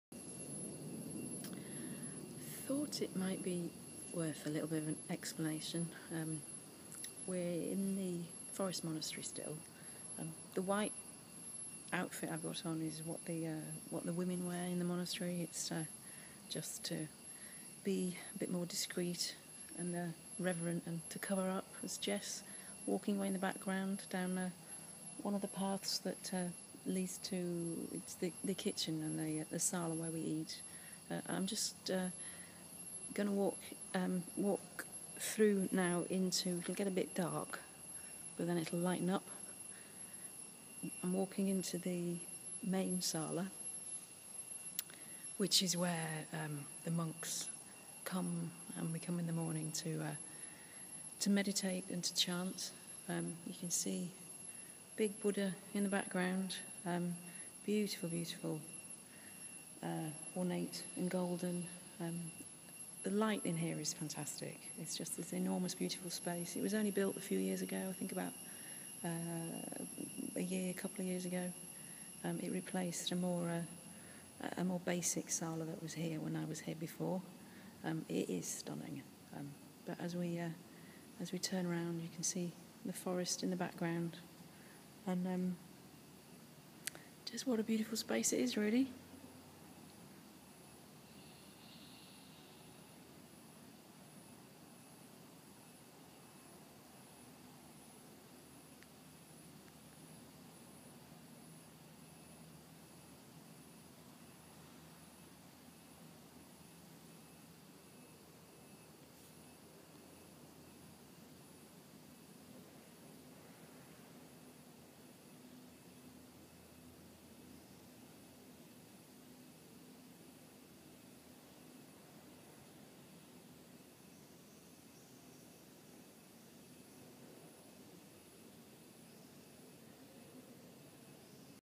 Tricia Walker & Benedict's Brother Thailand Film Recce
Tricia Walker, author of acclaimed Book of the Year novel Benedict's Brother, with scriptwriter Jessica Townsend in a Buddhist forest monastery in Thailand on location and research trip for the filming of the book, Sept 2011.